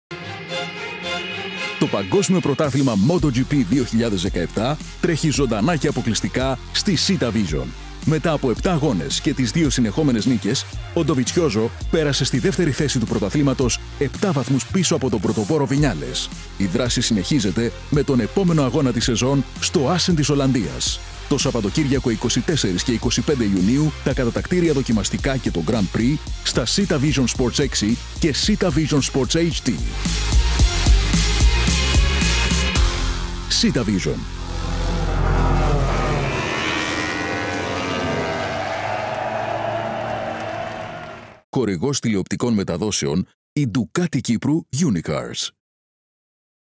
Moto GP Motul TT Assen 2017 24 και 25 Ιουνίου
Το Παγκόσμιο Πρωτάθλημα Moto GP 2017, τρέχει ζωντανά και αποκλειστικά στη Cytavision!
Η δράση συνεχίζεται με τον επόμενο αγώνα της σεζόν, στο Άσσεν της Ολλανδίας!